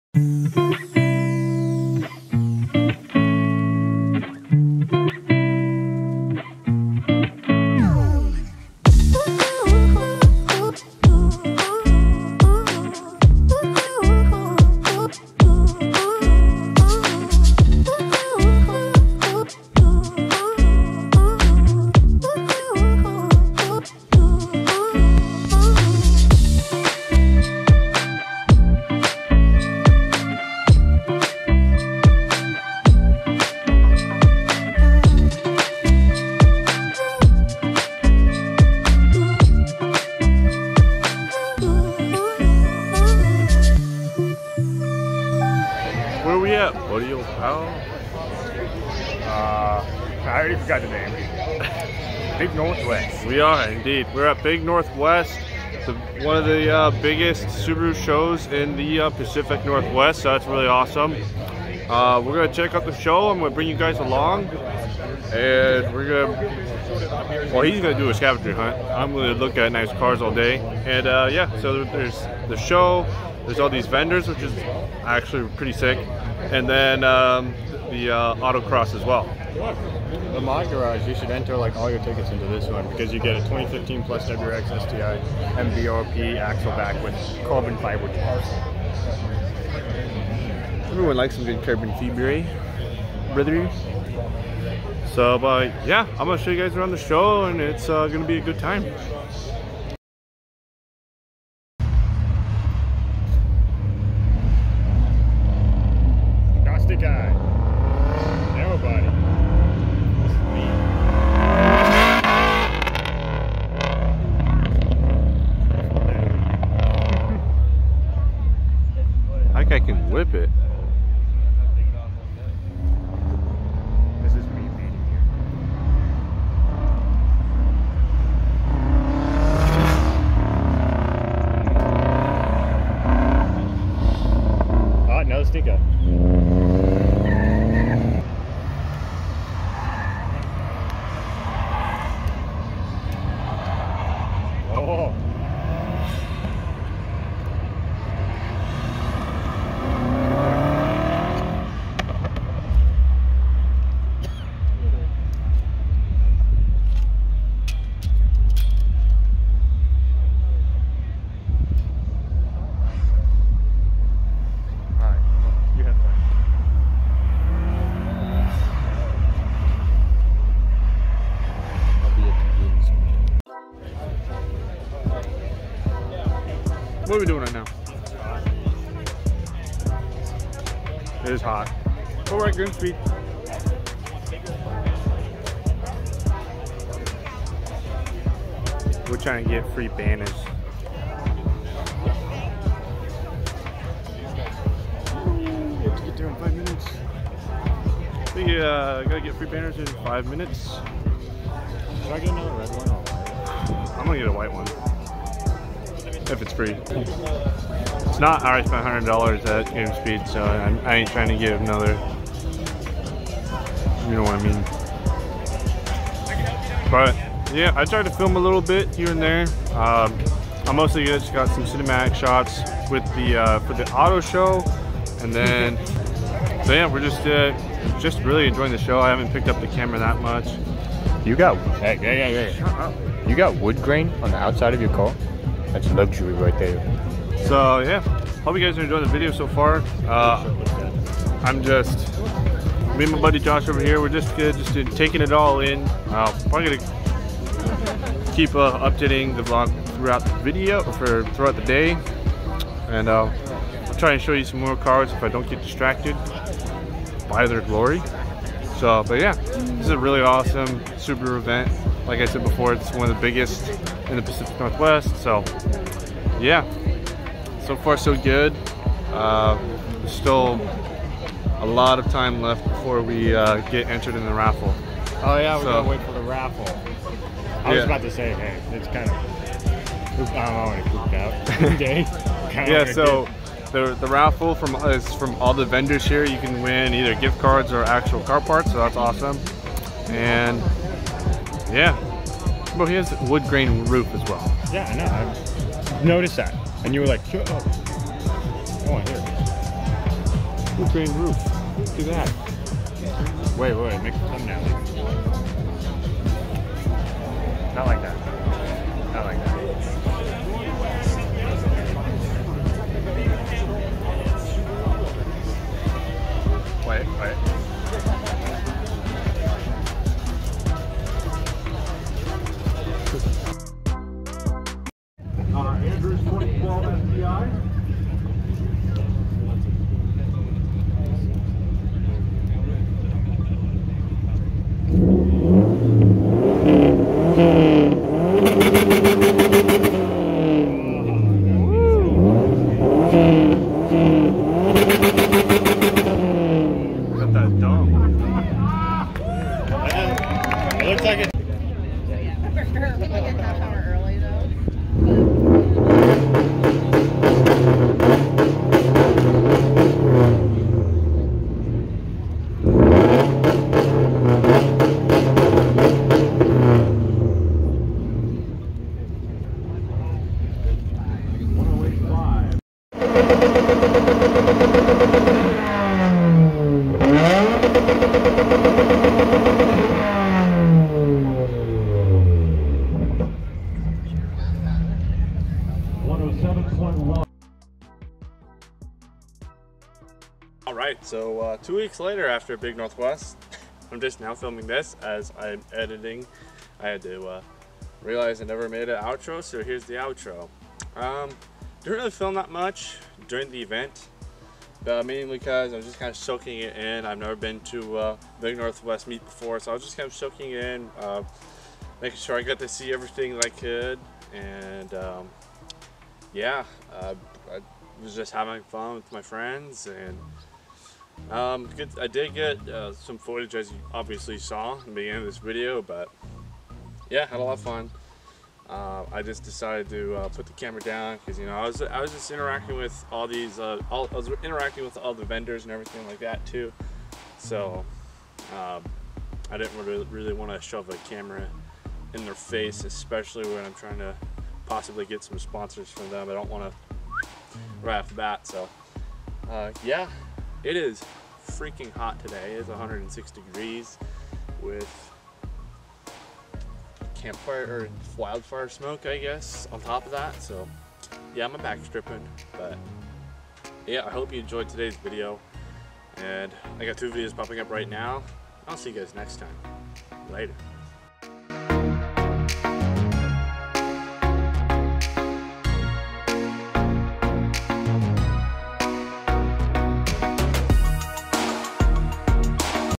BIG NW Subaru meet 2021! The BIGGEST Subaru Meet in the PNW!!
If you enjoy an entire video dedicated to Subaru's then this video is for you! even if you don't own one I want to see all of you there in 2022!